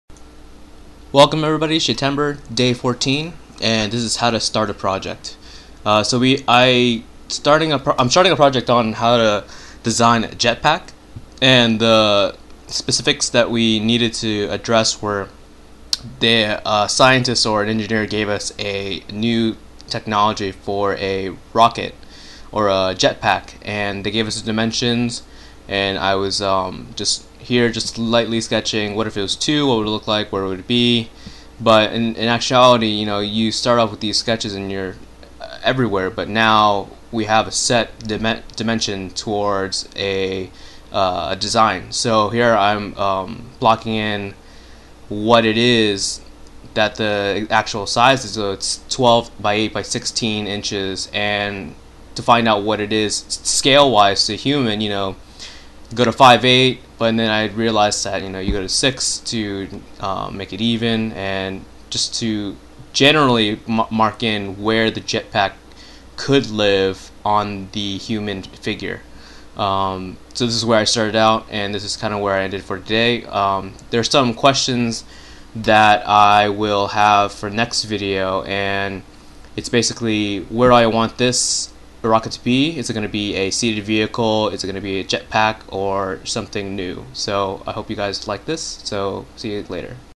Shaytember Day 14. Beginning a new project!
beginning a new project, a brief intro. Tomorrow will go more in depth of what i will be focusing on.